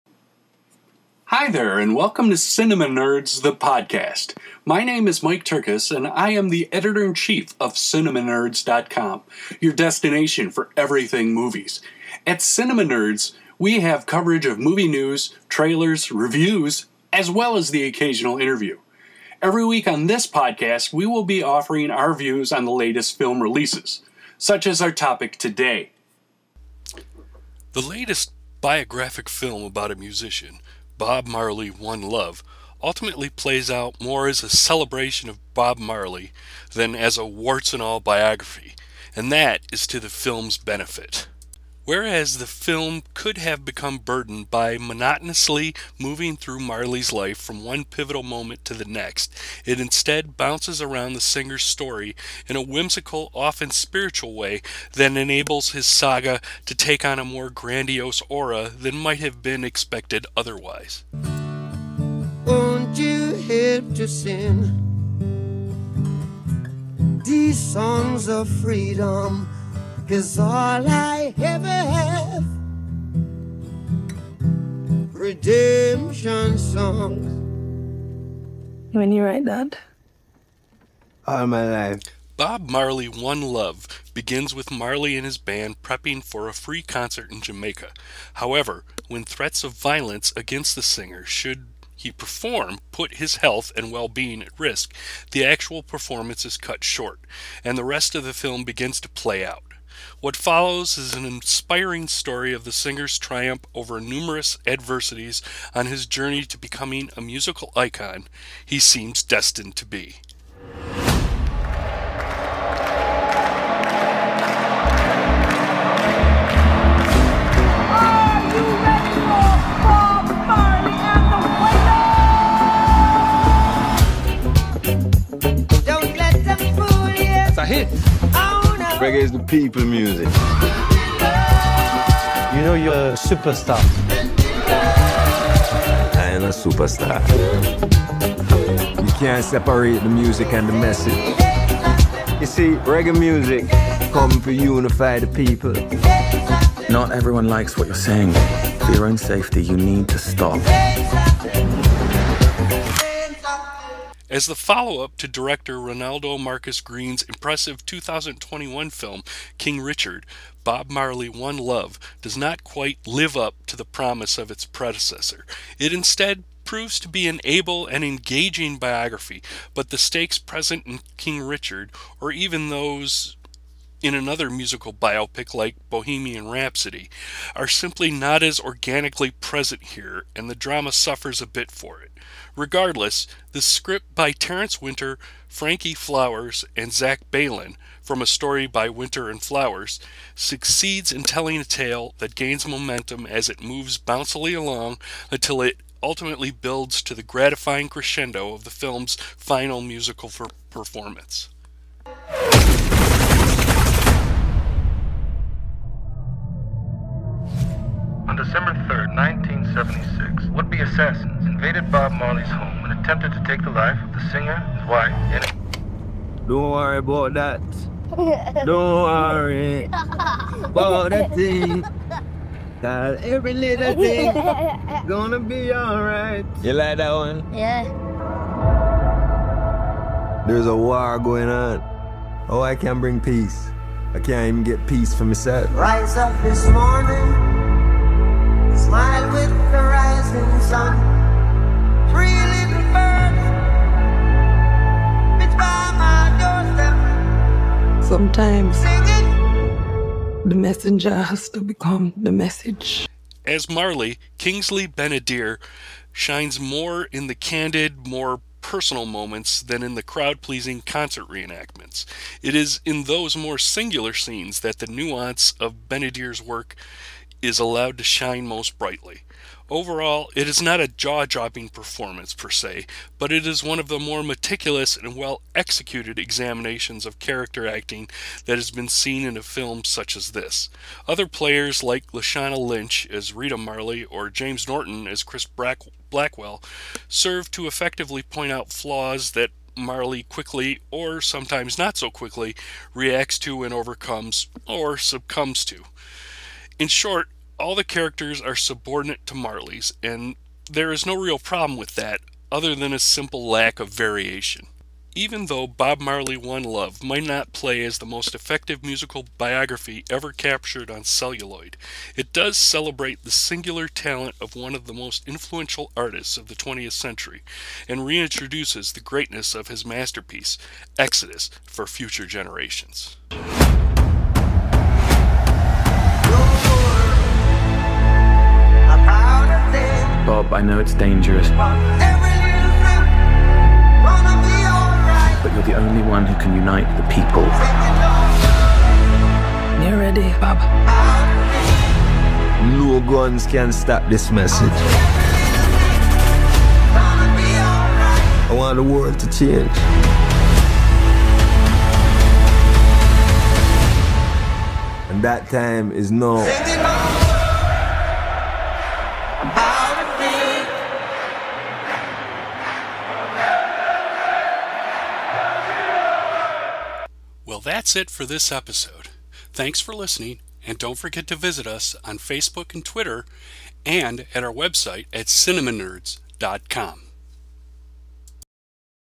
Episode 62: “Bob Marley: One Love”
In this episode of CinemaNerdz: The Podcast, Editor-in-Chief Mike Tyrkus says that the latest film about a musician – Bob Marley: One Love – ultimately plays out more as a celebration of Bob Marley than as a “warts-and-all" biography.

Check out the full CinemaNerdz review of Bob Marley: One Love